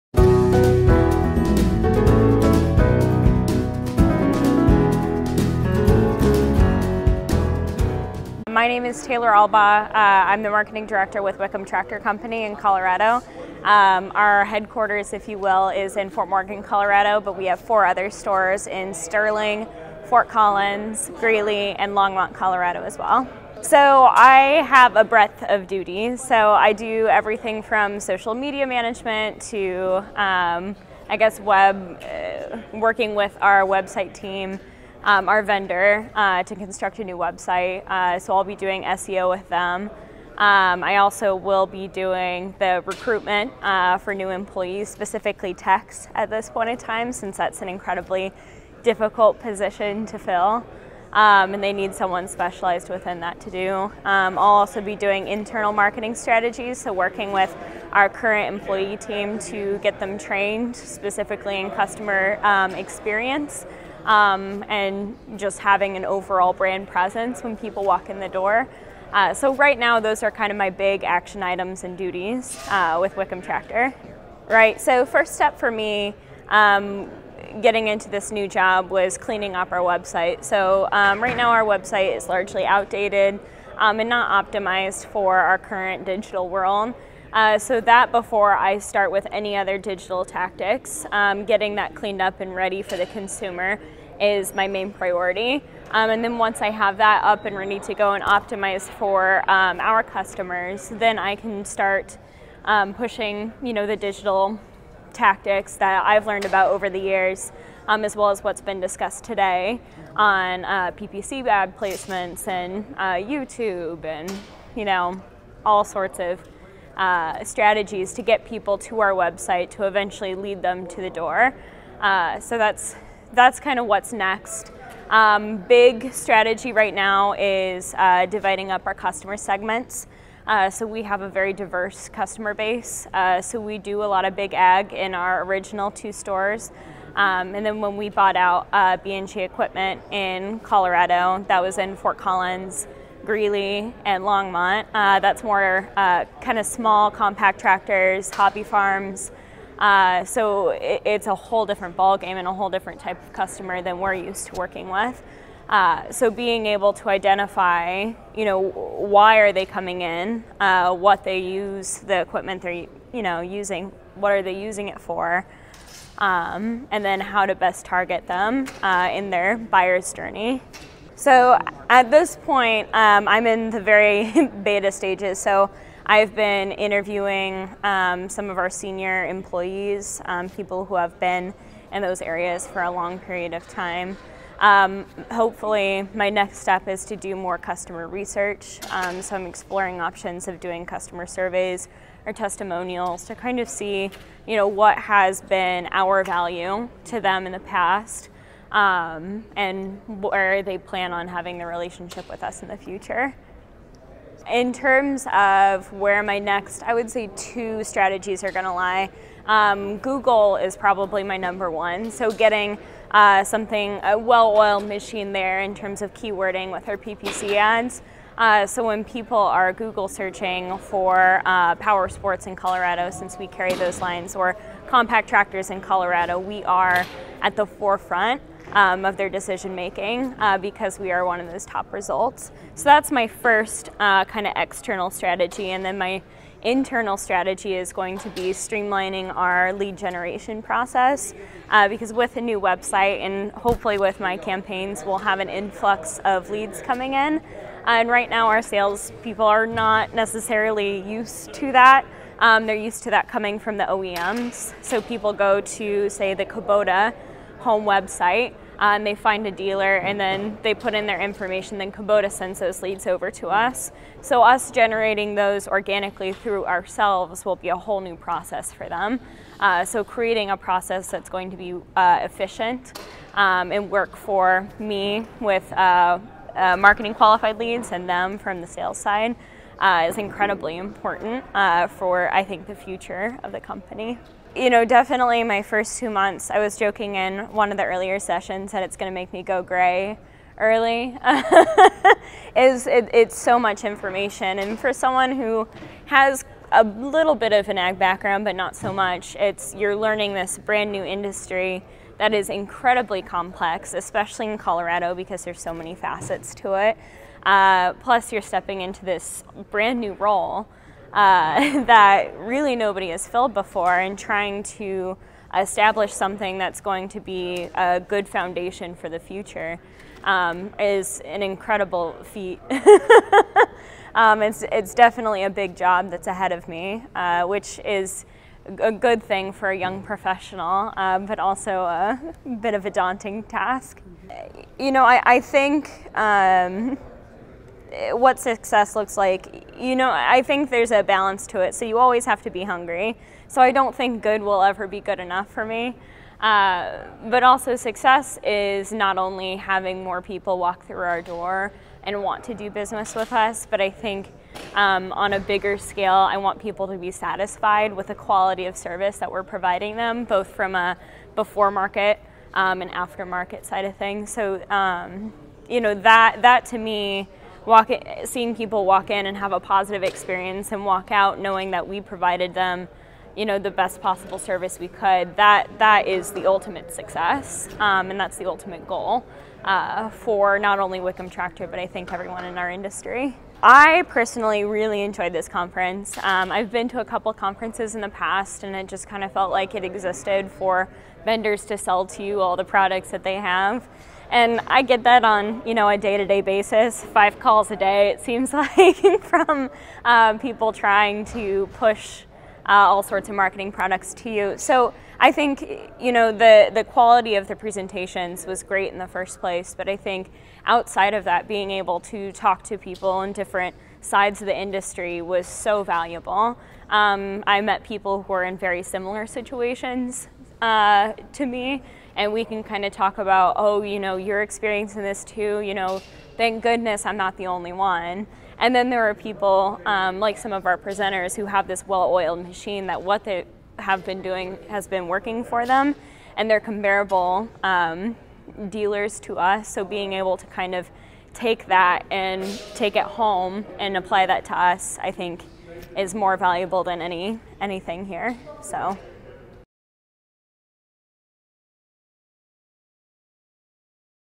Wickham Tractor: Integrating External & Internal Marketing
Taylor Albaugh is marketing director with Wickham Tractor, which has 5 locations and is based in Fort Morgan, Colo.. Rural Lifestyle Dealer caught up with Albaugh at the 2018 Dealership Minds Summit sponsored by Farm Equipment magazine. Albaugh comments on several marketing initiatives she is undertaking, including integrating internal and external marketing efforts.